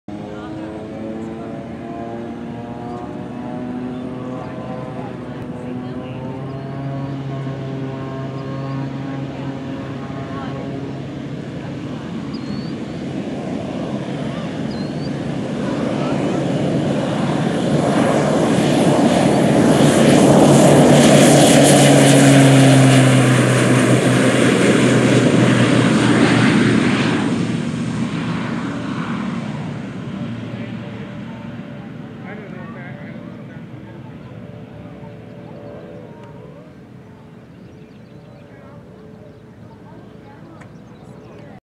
5/18/19 KVNW; SOCATA TBM-700 Takeoff
This nice little turboprop took off at 11:34 A.M.